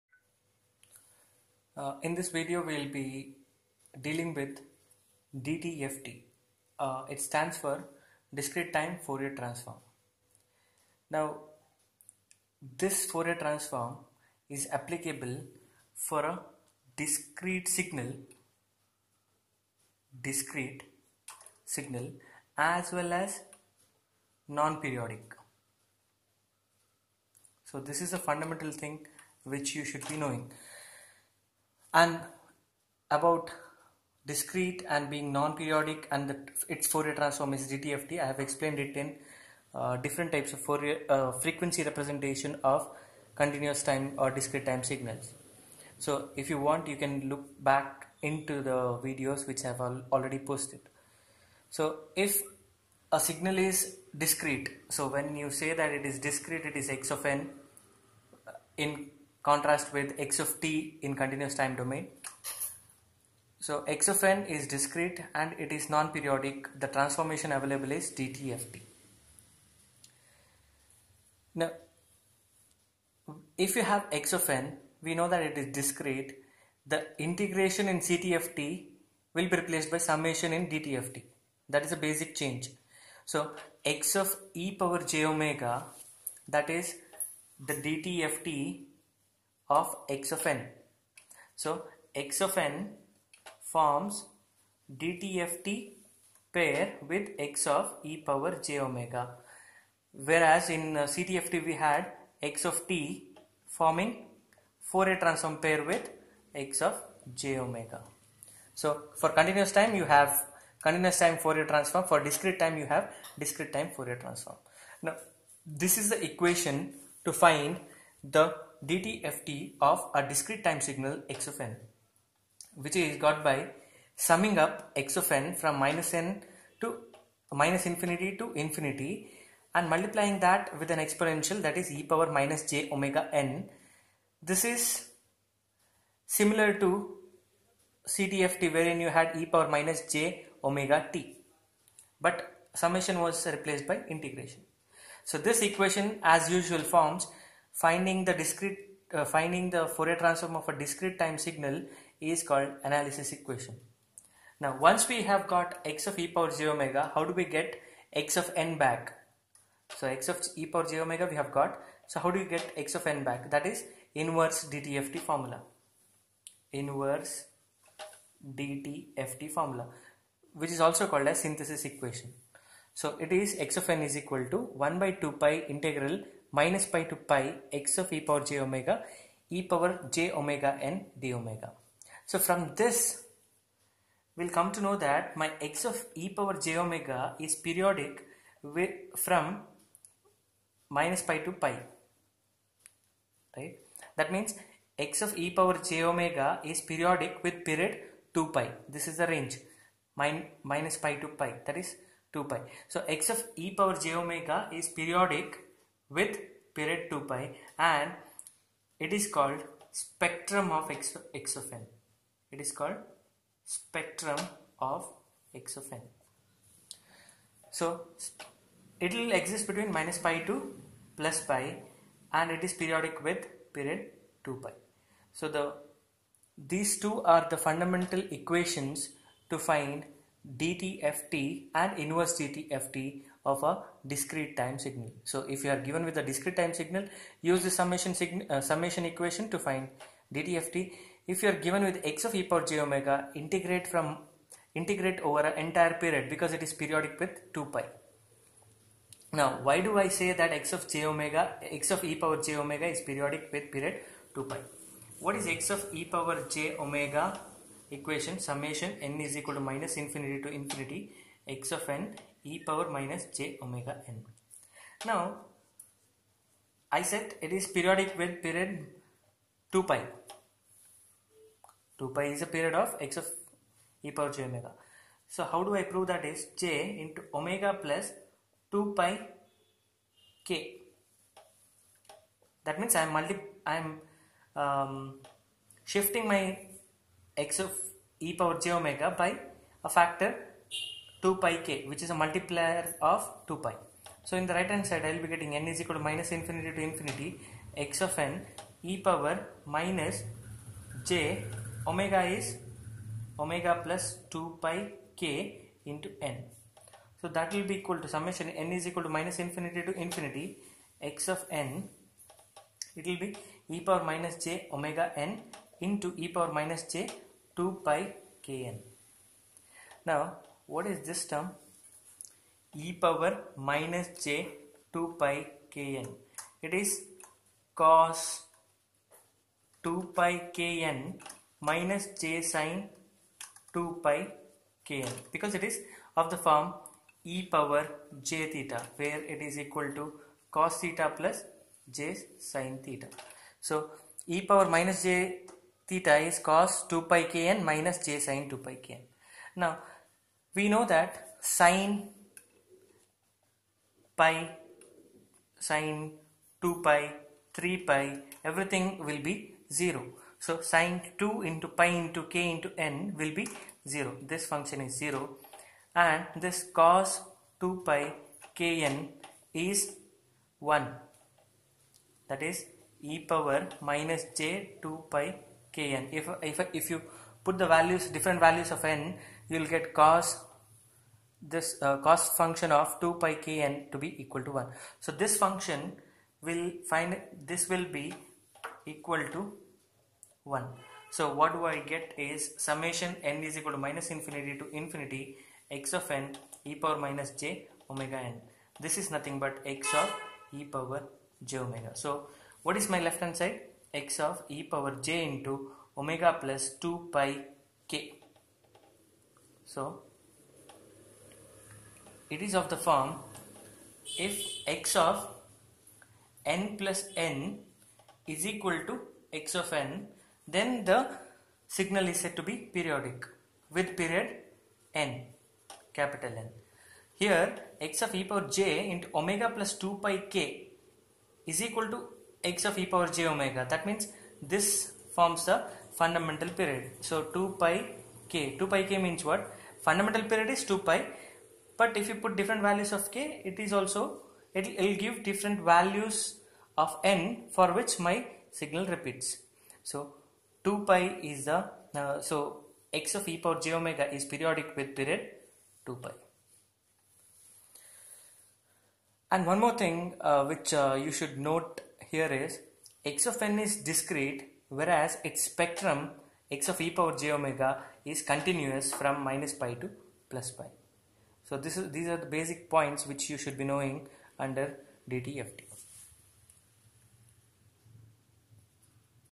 Introduction To Discrete Time Fourier Transform - DTFT
This video deals with basic equations which define DTFT .